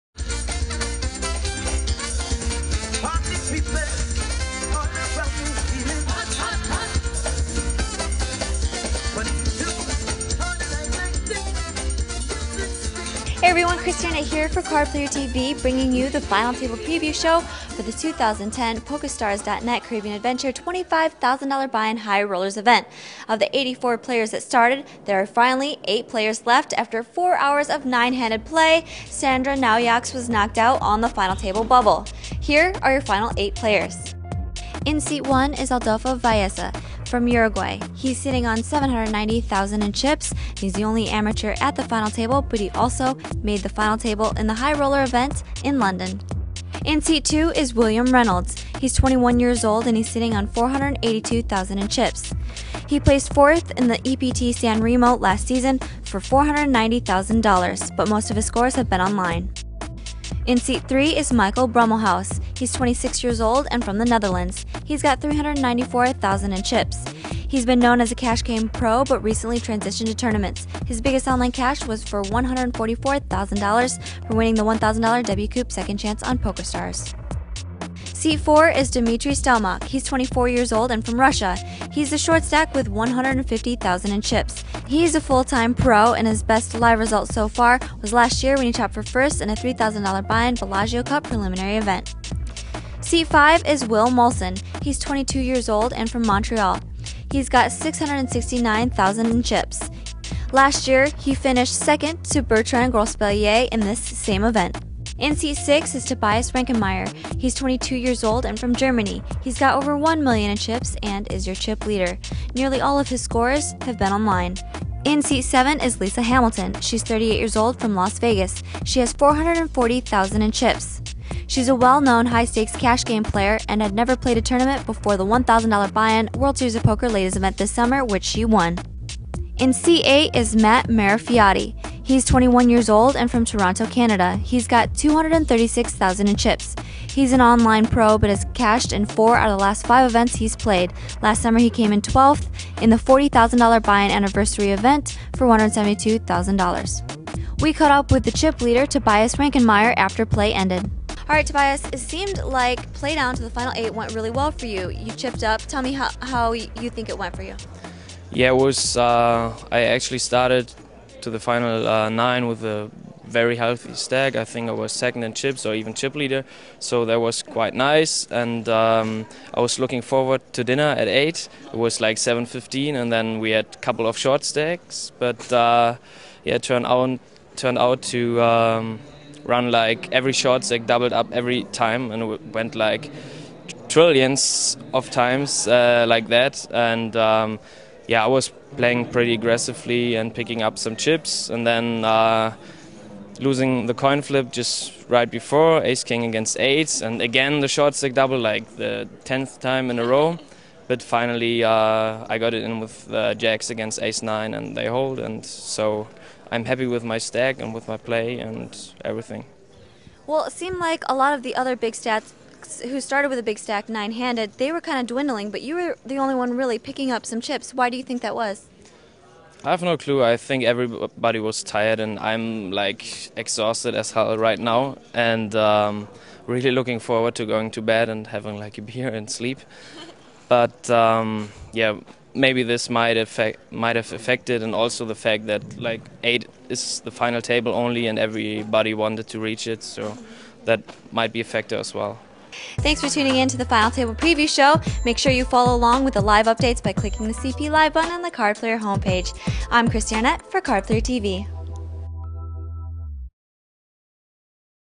2010 Poker Stars Caribbean Adventure High-Roller Event -- Final Table Preview
Card Player TV brings you a preview of the final table of the 2010 PokerStars Caribbean Adventure high-roller event final table, including an interview with chip leader Tobias Reinkemeier.